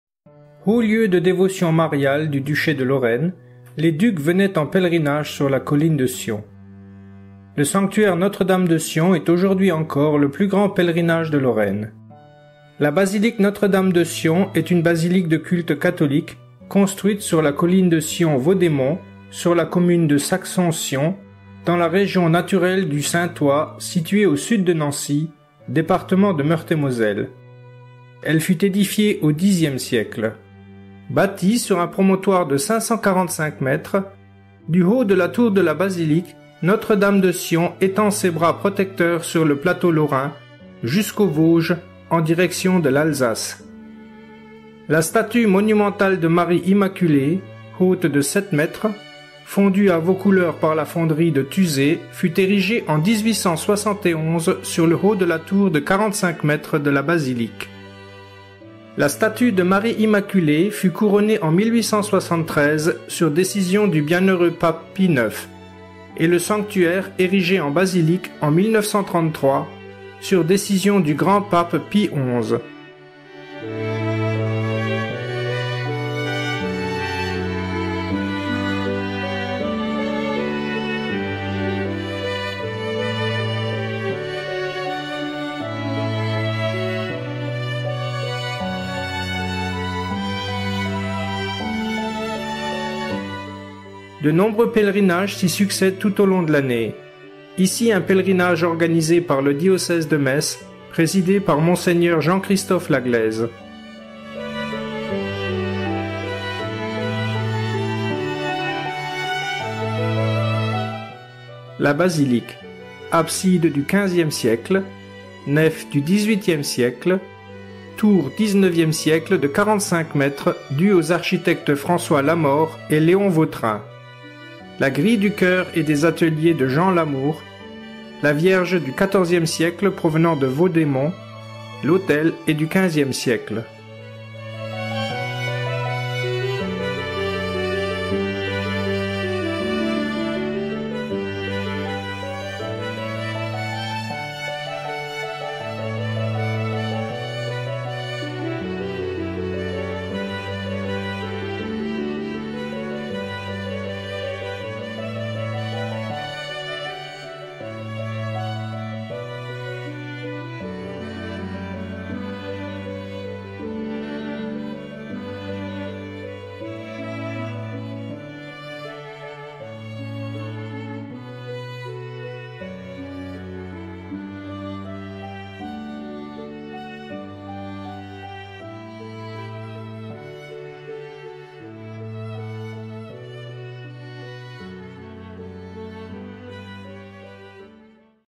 Sanctuaire Notre-Dame de Sion
La basilique Notre-Dame de Sion est une basilique de culte catholique construite sur la colline de Sion-Vaudémont, sur la commune de Saxon-Sion, dans la région naturelle du Saintois située au sud de Nancy (département de Meurthe-et-Moselle). Elle fut édifiée au Xè siècle. Haut lieu de dévotion mariale du Duché de Lorraine, les ducs venaient en pèlerinage sur la Colline de Sion. Le Sanctuaire Notre-Dame de Sion est aujourd'hui encore le plus grand lieu de pèlerinage lorrain.Il se compose de la Basilique Notre-Dame, du rameau des Soeurs Clarisses, de la chapelle St Joseph, de la Maison Notre-Dame ainsi que d'un accueil hébergement (pour retraite spirituelle, temps de silence). De nombreux pèlerinage s'y succèdent tout au long de l'année ; ici le diocèse de METZ présidé par Mgr Lagleize.Bâtie sur un promontoire de 545m, du haut de la tour de la basilique, Notre-Dame de Sion étend ses bras protecteurs sur le plateau lorrain, jusqu’aux Vosges, en direction de l’Alsace. La statue monumentale de Marie Immaculée, haute de 7 mètres, fondue à Vaucouleurs par la fonderie de Tusey fut érigée en 1871 sur le haut de la tour de 45 mètres de la basilique.
La statue de Marie Immaculée fut couronnée en 1873 sur décision du Bienheureux Pape Pie IX et le sanctuaire érigé en basilique en 1933, sur décision du grand Pape Pie XI.